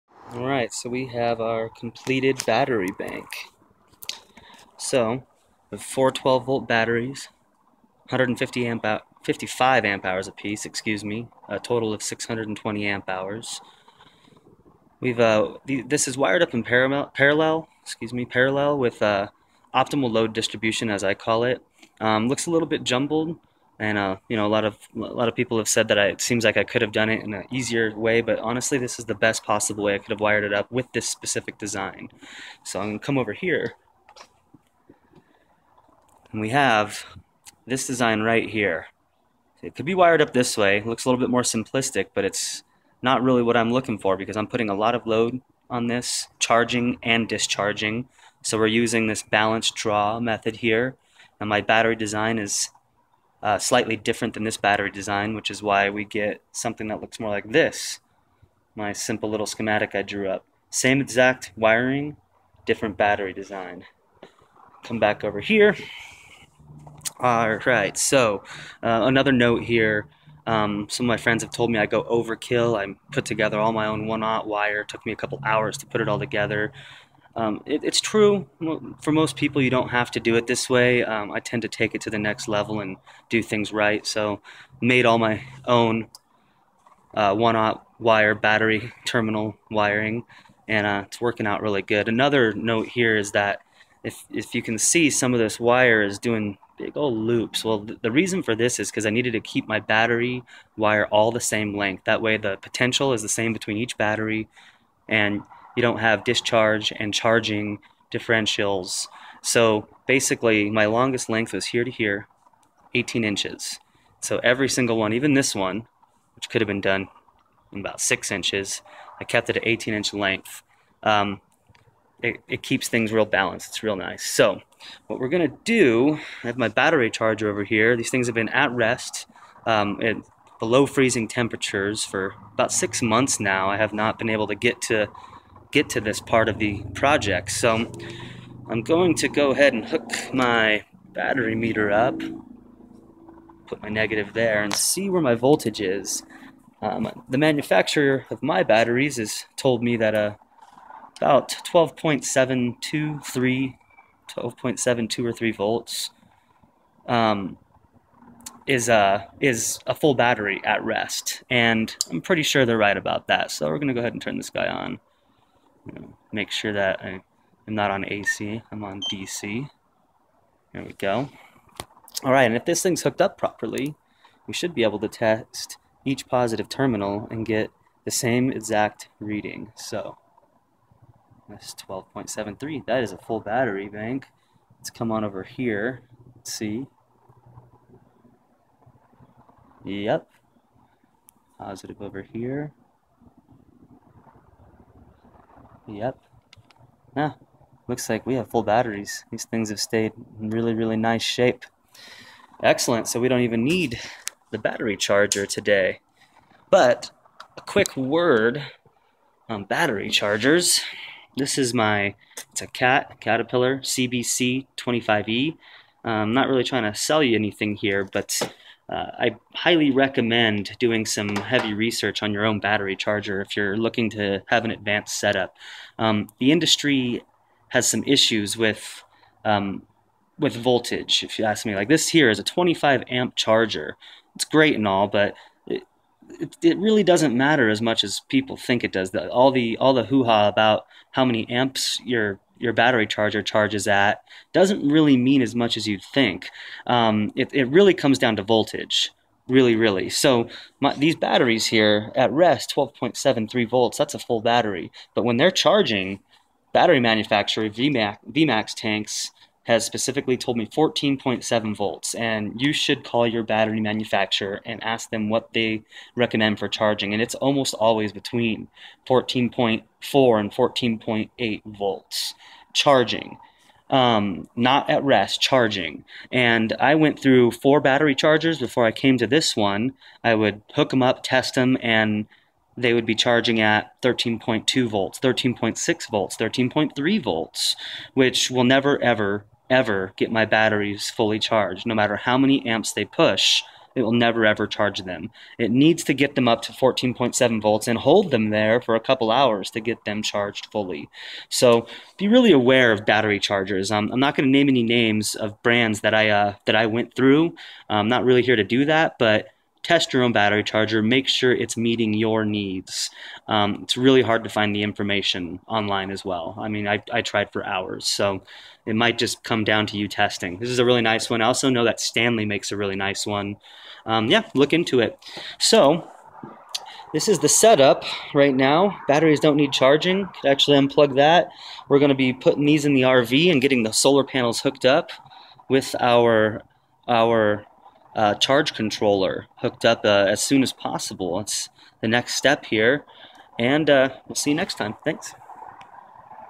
12v Battery bank set up in parallel with balanced draw
12v - 620Ah (155 Ah * 4) AGM (absorbed glass mat) sealed battery bank in parallel manufactured by Vmaxtanks and set up by yours truly.

These batteries have exceeded my expectations thus far and I cannot wait to get them set up with my RV solar charger and load distribution system. Testing in the video shows a full battery bank even after being at rest in sub zero temperatures for months. 12.73 volts is a full battery at rest for my batteries. A battery at rest should be left without charge or discharge for roughly an hour.

A note on battery chargers, make sure you understand some basic battery engineering. Average 12v batteries generally require 14.4 to 14.8 charging voltage (call your battery manufacturer). Check your battery charger's charging voltage by using a DMM while in operation. Many chargers are only reaching 13.2 to 13.8 and this will never get your batteries to full, regardless of what your idiot light tells you. Batteries need to have the proper charging voltage applied until the batteries themselves reach that voltage, and then hold that voltage for several hours before they are fully charged. Do some research, and set yourself up properly, or else you are not getting full potential out of your 12v system.

A note on battery monitors. This is another place where the industry lets us down. Cheap battery monitors such as the idiot lights in expensive RV's are generally hooked up somewhere in the wiring of your electrical system rather then directly to your batteries. Not only does this not account for the voltage drop problems in DC systems but also does not account for the voltage difference when charging and discharging. Be aware that many of these battery monitors are set to full at 13._ volts and as discussed this is not a full battery. these two issues with common battery monitors mean that many people are unknowingly destroying there batteries by only charging them to 55% to 70%. Ask yourself who profits from these issues? Certainly not the consumer.

The best way to check your battery level is physically, by opening it up and checking the fluid density with a hydrometer (10 bucks at an auto part store). This works great for standard lead acid batteries which is cost effectively the best way to go. I have my batteries indoors so must use sealed AGM batteries, no way to use a hydrometer on this set up. I must rely on DMM's and a microprocessor digital battery monitor for decently reliable readings.

A great link to related information on real 12 volt tips from an engineering perspective, not a sales pitch: